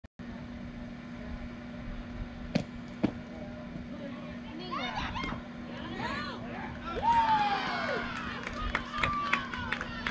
Another perfect throw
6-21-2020 - USFA Regional State 14C, Brownsburg, IN, Bracket play game 4
Summit City Sluggers 14u vs River City Outlaws 06